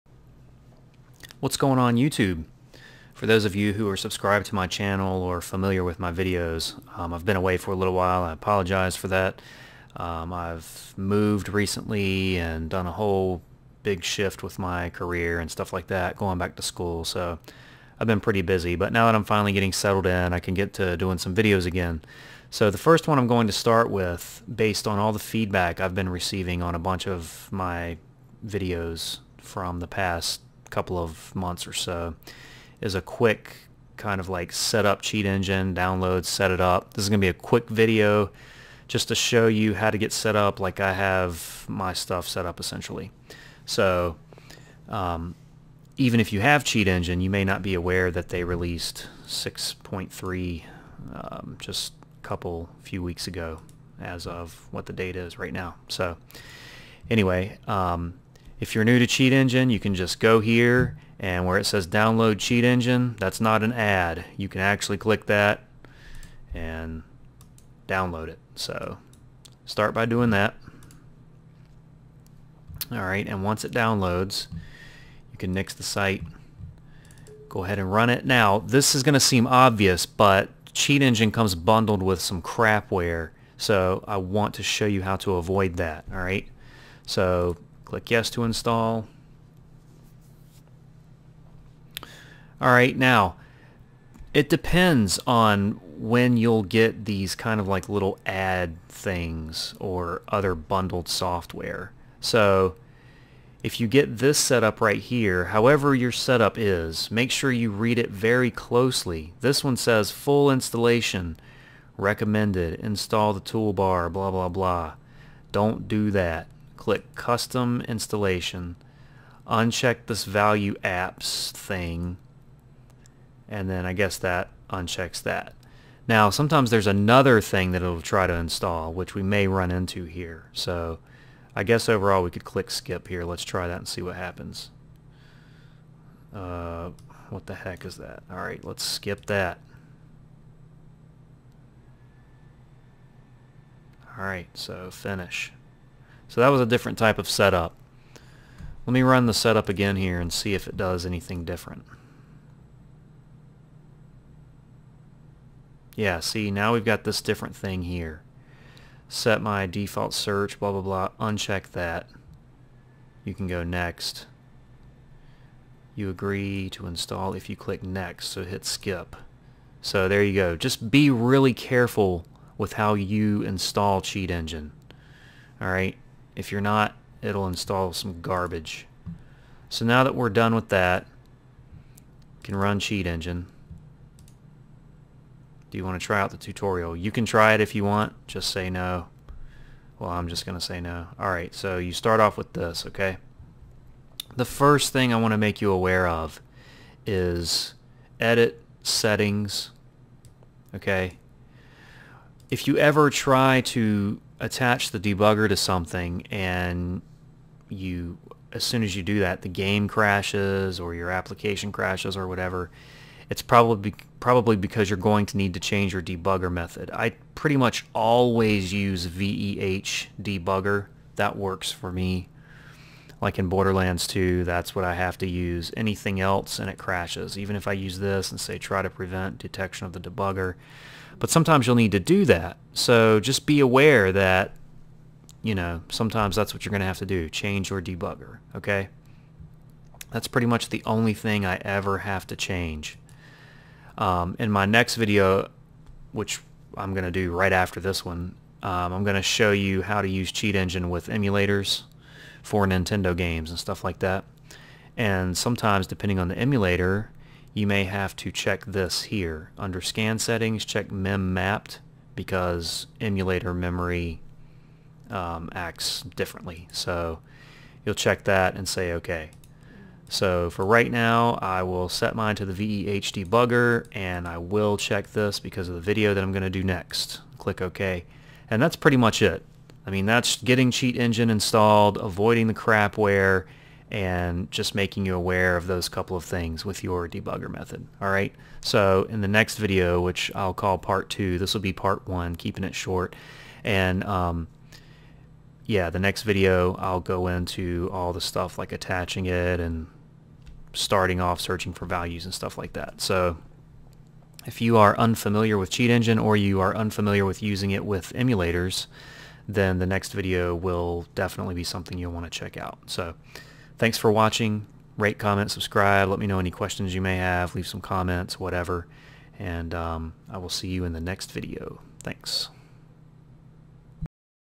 Cheat Engine 6.4 Tutorial Intro: Installation and Quick Tips
In this video, I explain the installation of Cheat Engine and a few important quick tips to remember going forward. This is the first in a series of many in which I teach the very basics of Cheat Engine, then continue building on that knowledge. =)